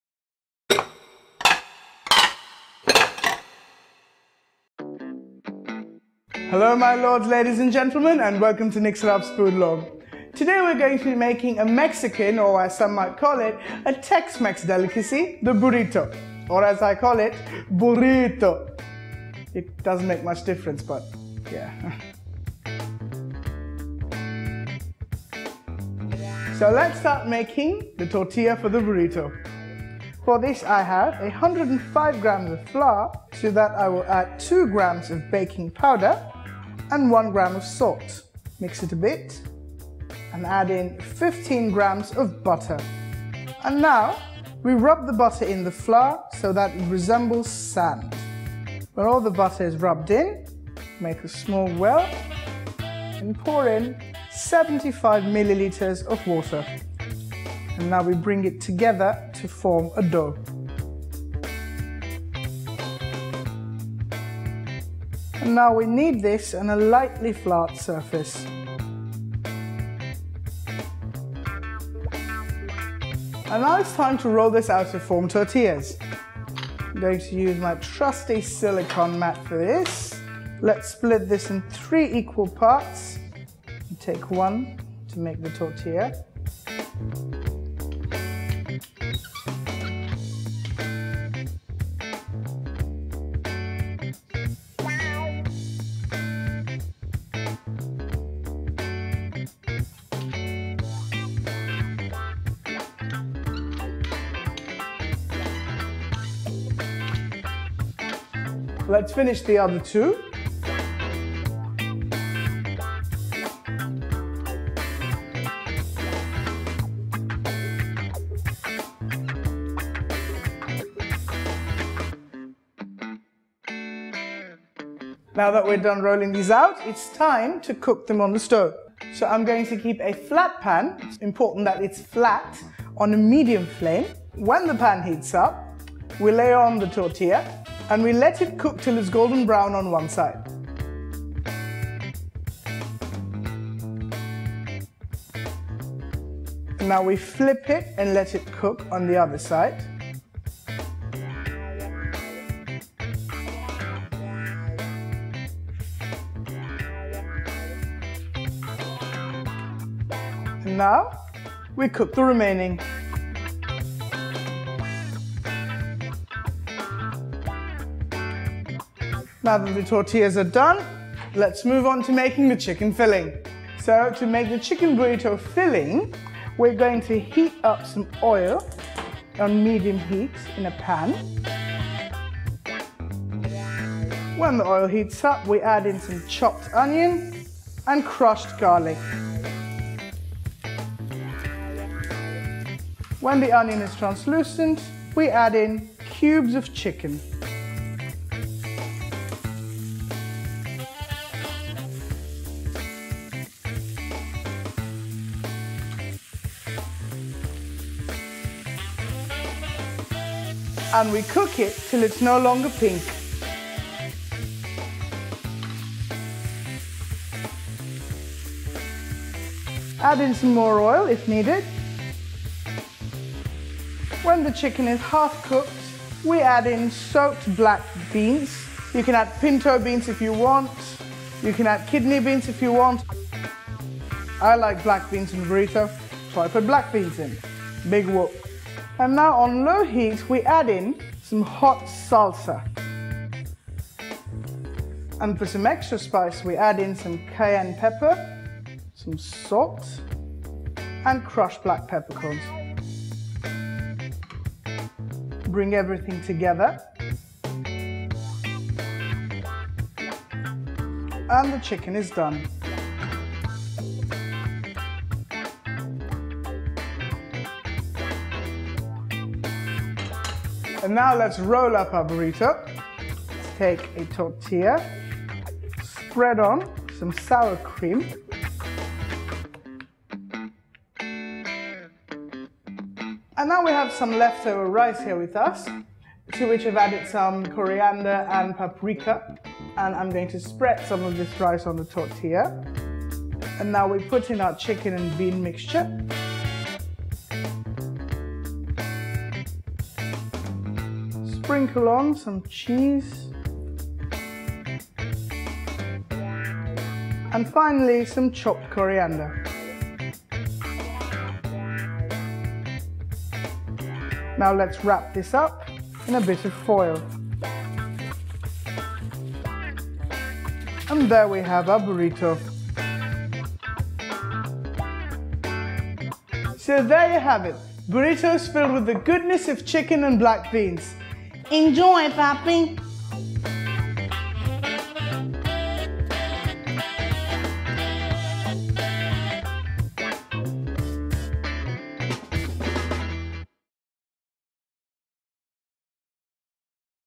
How To Make Burrito | Homemade Burritos Recipe | Perfect Food For Camp | Nick Saraf's Foodlog
Learn how to make Burrito at home with Chef Nick Saraf only on Get Curried

Call it Burrrritttooo or  Burrito it tastes amazing both the way! So make one for yourself with the help of chef Nick Saraf only on Get Curried

You can certainly do better than cereal for your camping breakfast. These make-ahead burritos travel well in your cooler or camping fridge, and they taste great with tea or coffee!!!

Ingredients:
For Tortilla
105gms of flour
2gms of baking powder
1gm of salt
15gms of butter
75ml of water

Chicken Filling
- Oil
- Chopped Onion
- Crushed garlic
- Cubes of Chicken
- Soaked black beans
- Hot Salsa
- Cayenne Pepper
- Salt
- Crushed black pepper corns

Assemble
- Salad Cream
- Left over rice mixed with coriander & paprika
- cheese
- coriander leaves

Method:
- Make dough by adding flour, baking powder, salt, butter mix it well and then add water.

- On a flat surface/silicon mat,  roll the dough balls to form tortilla.

- Keep a pan on medium flame and heat the Tortilla till its golden brown on one side. Flip it and cook in on the other side.

- Heat some oil on a medium flame, add chopped onions and crushed garlic cook until they turn translucent

- Add cubes of chicken cook it till the chicken is no longer pink add more oil if needed

- Add soaked black beans let them cook.

- On low heat add hot salsa, cayenne pepper, salt and black pepper

Assemble
- On a tortilla spread some sour cream, some rice,chicken and bean mixture.

- Sprinkle some cheese and chopped coriander.

- Wrap it in a foil & the Burrito is ready!

Host: Nick Saraf
Copyrights: REPL